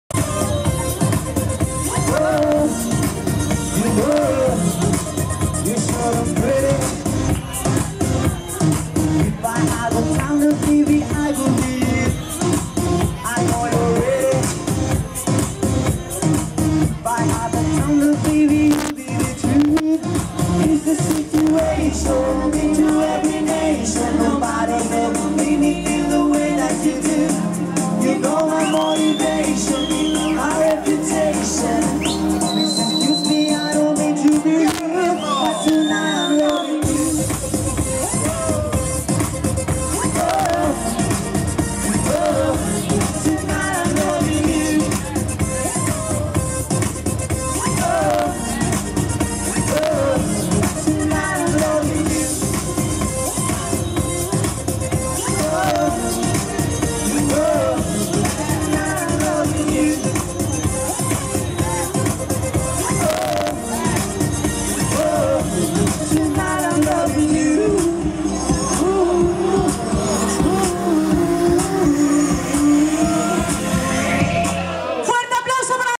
Boda en Brisas del Mar en Piles con la Orquesta Diamonds. Musica para tu boda. Canciones de boda
La Orquesta Diamonds estuvo en Brisas de Mar en Piles actuando en una boda muy divertida. Si quieres ver más videos puedes entrar